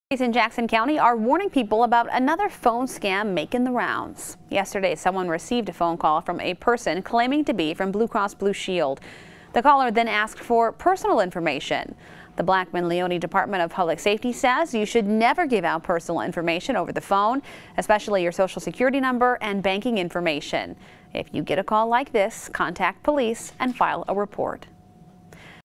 Another phone scam in Jackson County
Authorities in Jackson County are warning people about another phone scam making the rounds. Someone received a phone call from a person claiming to be Blue Cross Blue Shield. The caller then asked for personal information.  The Blackman Leoni Department of Public Safety says you should never give out personal information over the phone, especially your social security number and banking information. If you get a call like this, contact police and file a report.